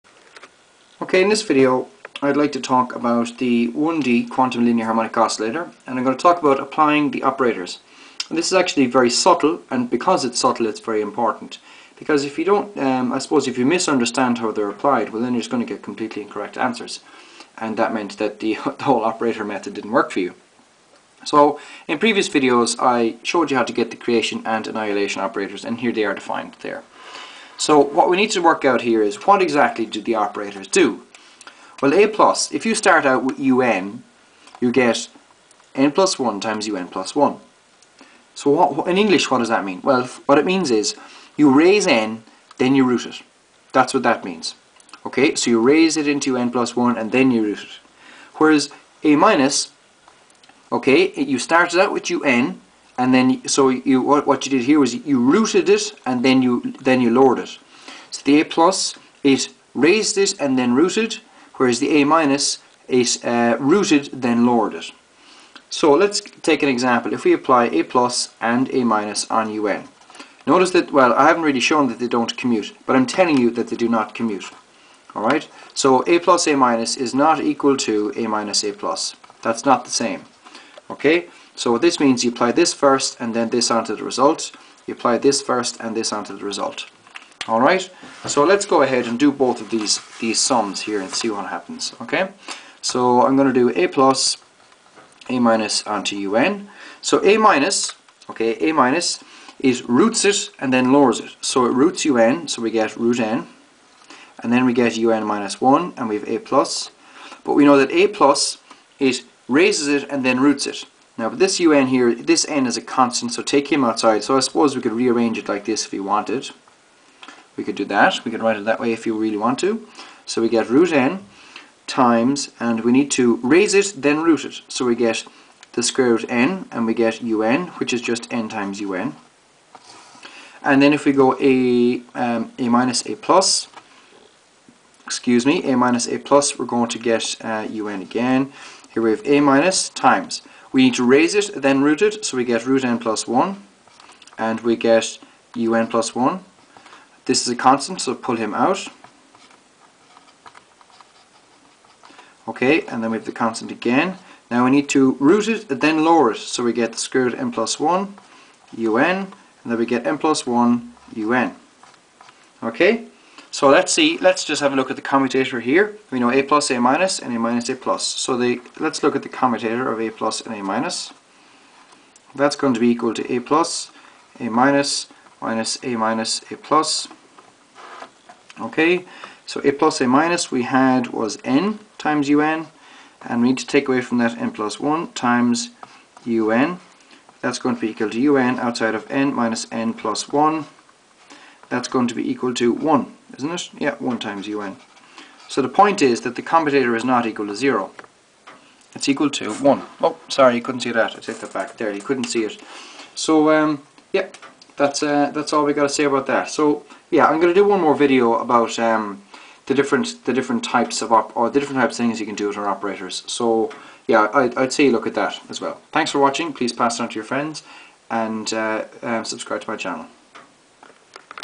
Quantum LHO 6 : Applying Ladder Operators
In this video I show you how to use the ladder operators of the LHO (it's perhaps not as simple as it looks and there are one or two subtleties)

Quantum Linear Harmonic Oscillator